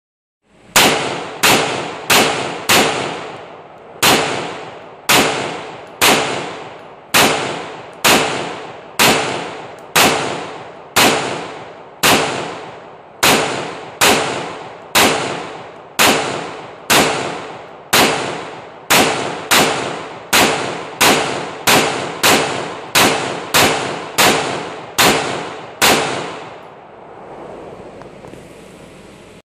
Shooting the Core15 AR15
Popping off some rounds.